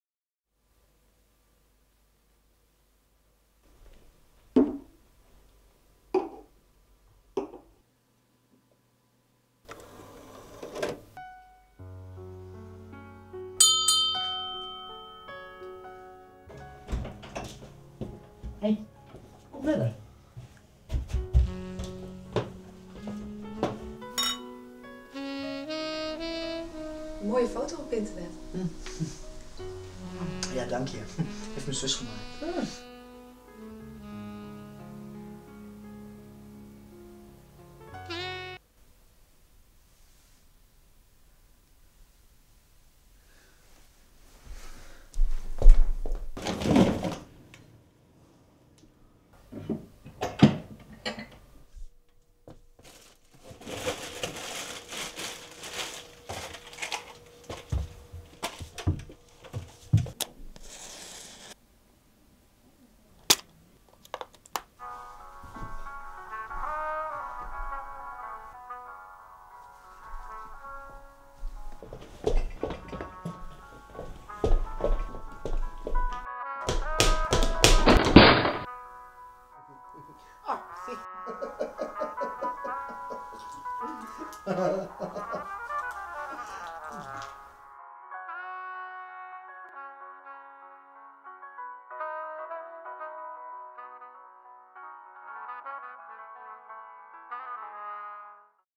Henk is er klaar voor....
Henk heeft meer in huis dan hij denkt...de (viral) commercial voor de Hulpverleningsdienst Groningen. Bekijk deze commercial en ga snel naar
Muziek door Remco Wind